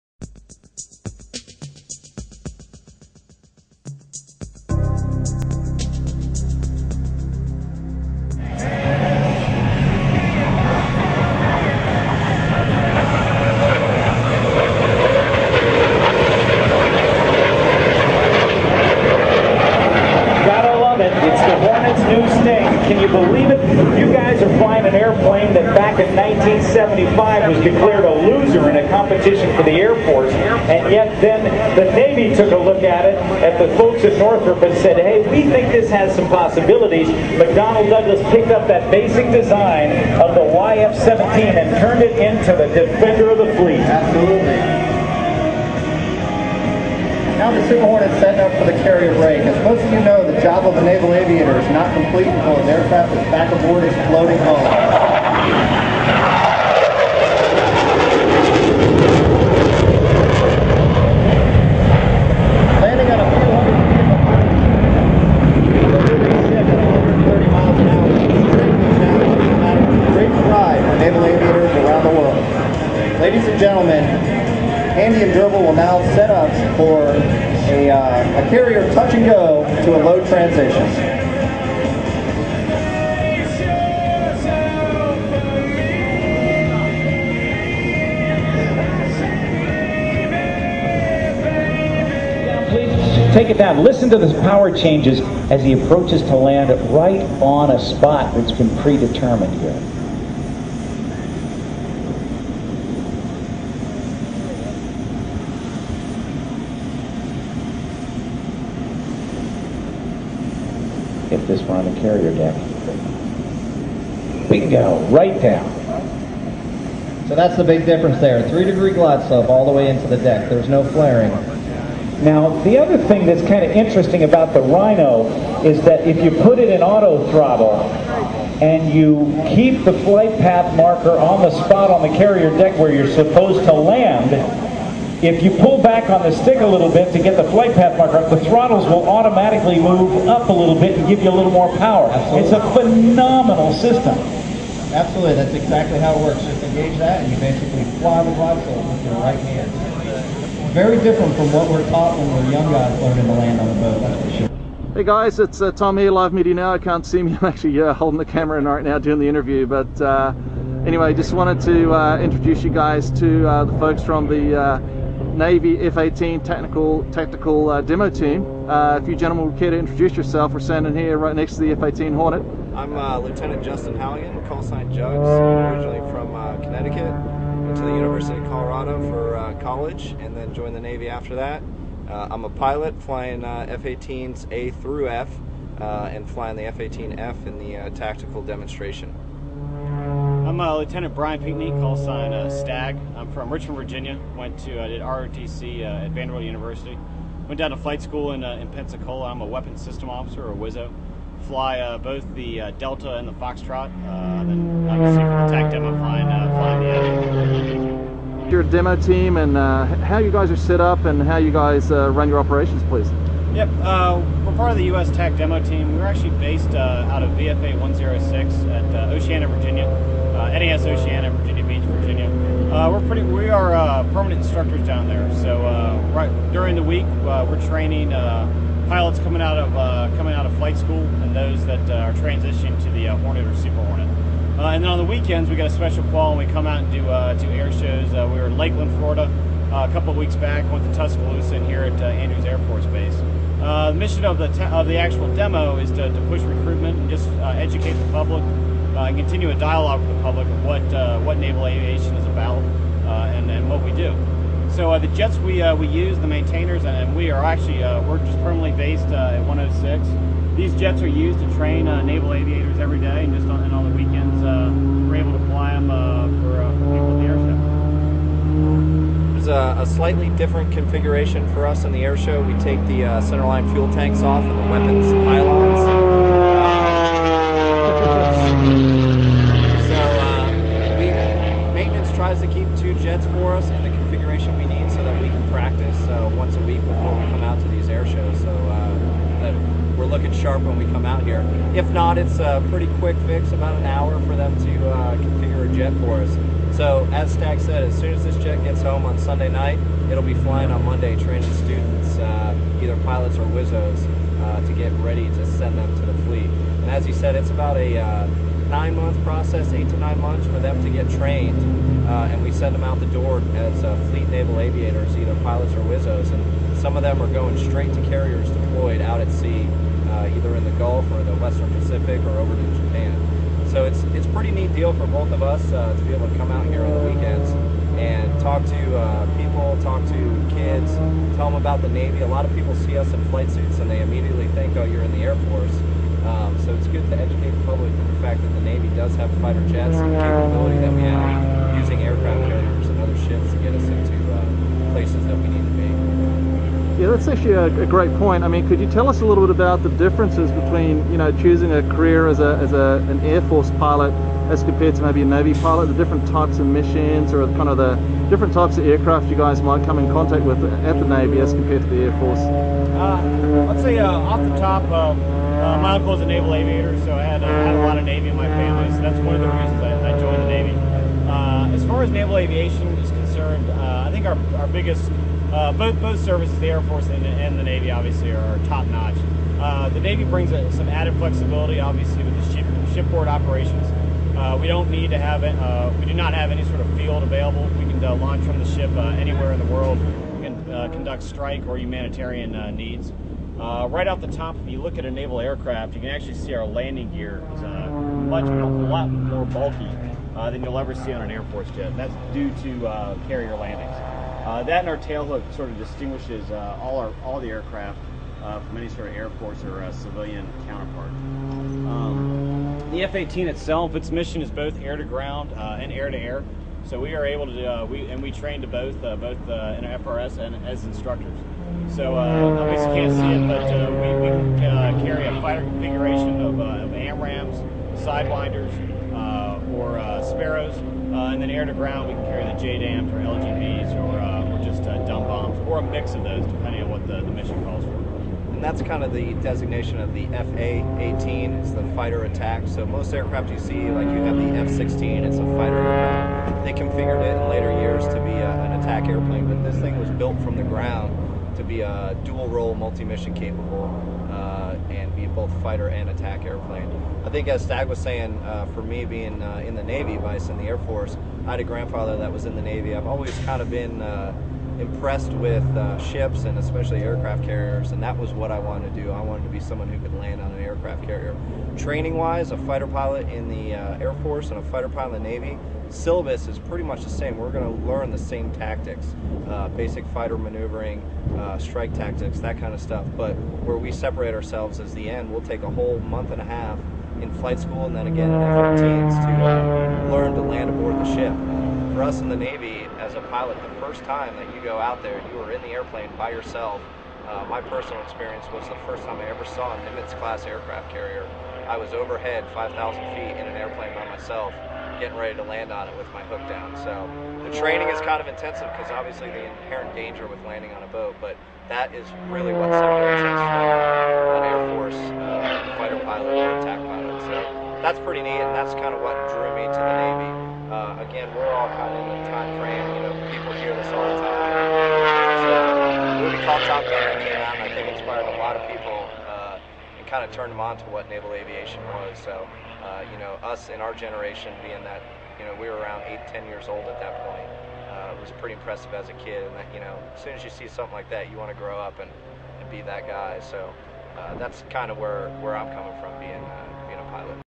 F18 Super Hornet at Andrews JSOH May 2009
The USN F-18 Tactical Demo team talking with Tom of Live Media Now on the ramp with their jet.